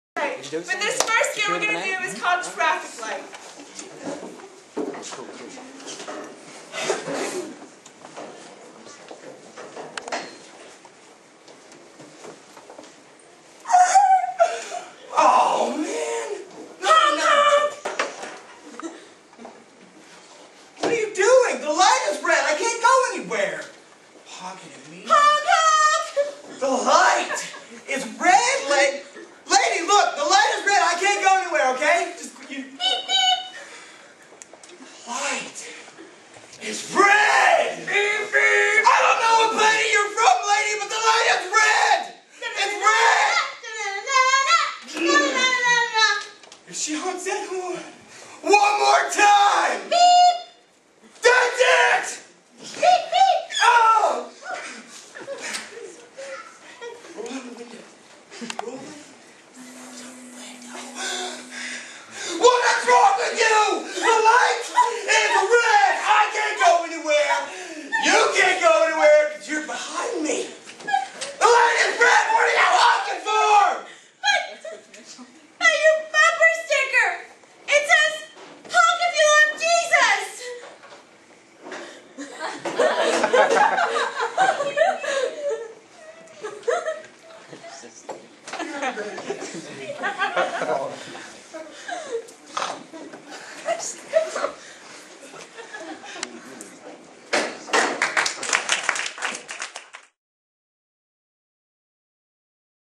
New Life Drama Company: Traffic Light
Short Skit called "Traffic Light" from a Drama team called: "New Life Drama Company" that visited my church, Living Springs Baptist.